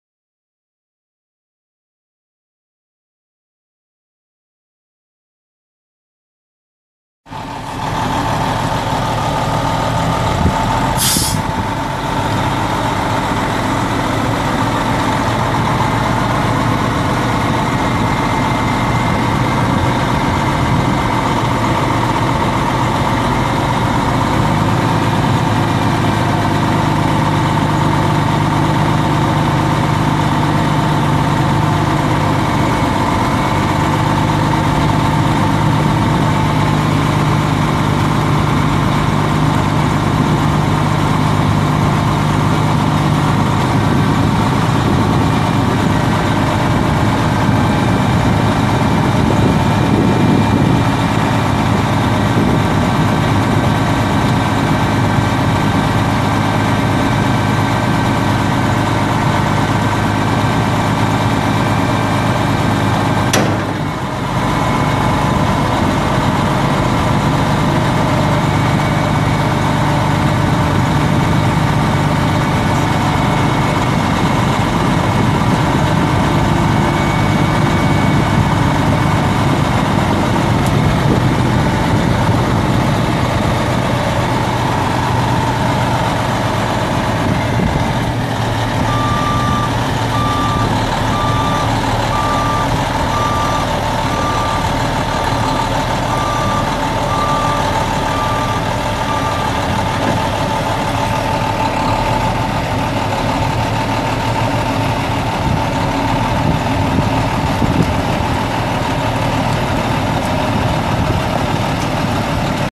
1998 International Paystar 5000 mixer truck for sale | no-reserve Internet auction June 9, 2016
1998 International Paystar 5000 mixer truck
228,719 miles on odometer
Cummins M11 10.8L L6 diesel engine
280 HP
Nine speed manual transmission
Lo and Lo-Lo gears
Tandem axle
AC and heat
Heated mirrors
Air suspension seat
McNeilus 11 cu. yd. mixer
Rear drum controls
Drum/axle controls
(3) steel chute extensions
125 gallon water tank
Air brakes
425/65R22.5 front tires
11R22.5 rear tires
Bridgemaster 13,200 lbs pusher axle
Kansas title
1HTTWAHT1WJ000479